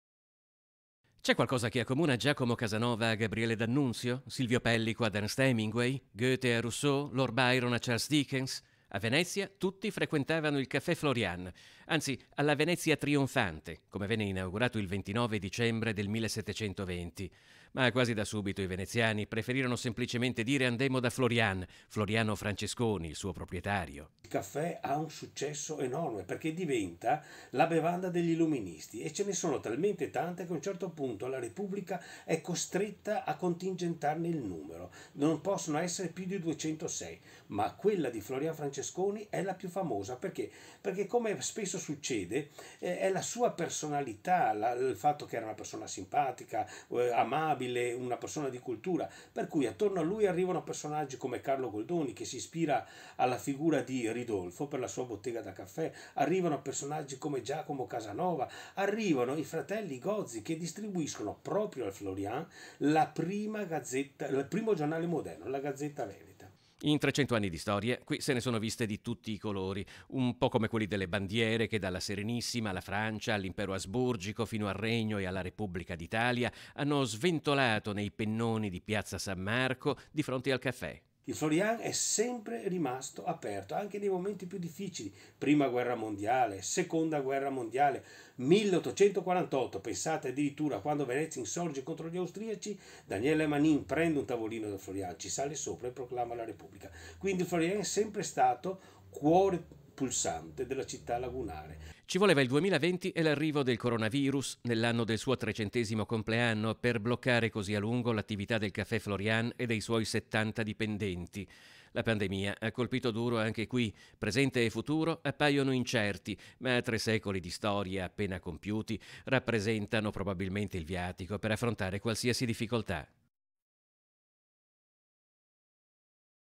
IL TRECENTESIMO COMPLEANNO 'IN QUARANTENA” DEL CAFFE’ FLORIAN | 29/12/2020
A3 NEWS Venezia 29/12/2020 - VENEZIA - Il Caffè Florian di Piazza San Marco compie 300 anni. Un compleanno che con la chiusura forzata per Coronavirus arriva in uno dei momenti più difficili della sua storia.  || C’è qualcosa che accomuna Giacomo Casanova a Gabriele D’Annunzio? Silvio Pellico ad Ernest Hemingway? Goethe a Rousseau? Lord Byron a Charles Dickens?A Venezia tutti frequentavano il Caffè Florian anzi “Alla Venezia Trionfante”, come venne inaugurato il 29 dicembre del 1720. Ma da subito i veneziani preferivano semplicemente dire“andémo da Floriàn”, Floriano Francesconi il suo proprietario.In 300 anni di storia qui se ne sono viste di tutti i colori. Un po’ come quelli delle bandiere che dalla Serenissima alla Francia all’Impero Asburgico fino al Regno e alla Repubblica d’Italia hanno sventolato dai pennoni di Piazza San Marco di fronte al Caffè.Ci voleva il 2020 e l’arrivo del Coronavirus nell’anno del suo trecentesimo compleanno per bloccare così a lungo l’attività del Caffè Florian e dei suoi 70 dipendenti. La pandemia ha colpito duro anche qui. Presente e futuro appaiono incerti ma tre secoli di storia appena compiuti rappresentano probabilmente il viatico per affrontare qualsiasi difficoltà. - Intervistati STEFANO STIPITIVICH (COORDINATORE ATTIVITA’ CULTURALI FLORIAN) (Servizio di Filippo Fois) - Segui Antenna Tre anche sul digitale terrestre! Visita il sito